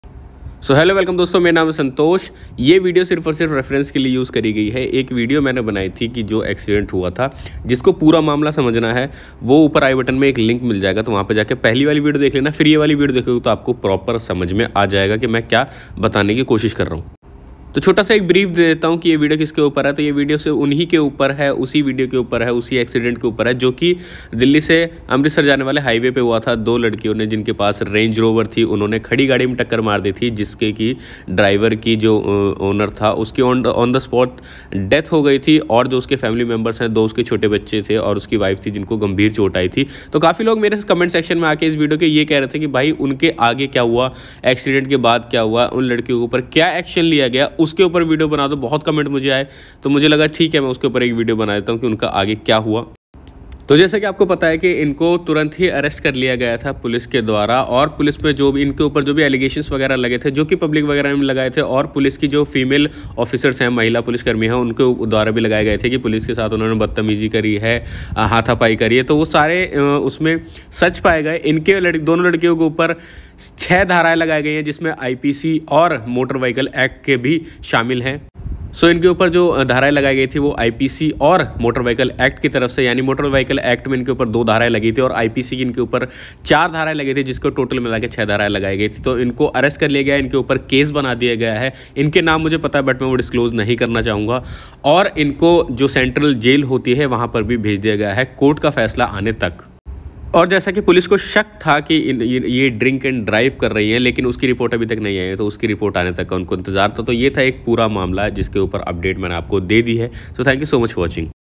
Final Update on Range Rover Girls | गयी JAIL 🚨 Range Rover Accident Ambala | Part - 2
A Palampur resident, Mohit Sharma, died in the accident, while his wife, Deepti, and two daughters were injured.

Following the accident, the SUV driver, identified as Variyata Janglan, 26, a resident of Panipat, was arrested and sent to Central Jail, Ambala in judicial custody.

Thankyou 😊

About me:
Hi, Myself Santosh Kushwaha
From Kanpur U.p (UP78)